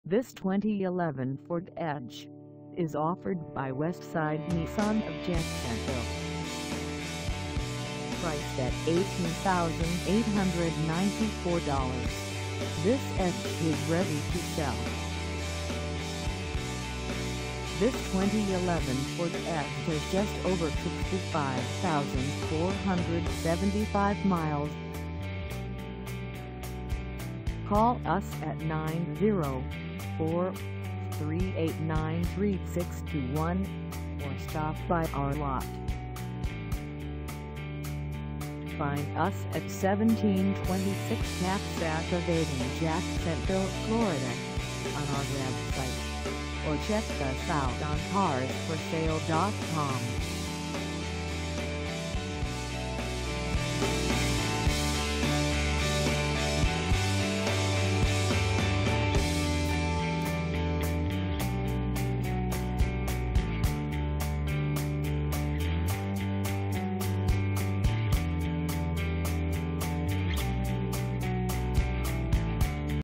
2011 Ford Edge SEL 4dr SUV for sale in Jacksonville, FL 3221
This 2011 Ford Edge SEL 4dr SUV is for sale in Jacksonville, FL 32210 at WESTSIDE NISSAN OF JACKSONVILLE.

Engine: 3.5L V6
Transmission: Automatic 6-Speed
Color: Black
Mileage: 65475
Contact WESTSIDE NISSAN OF JACKSONVILLE 1726 CASSAT AVE Jacksonville, FL 32210 to test drive this 2011 Ford Edge SEL 4dr SUV today.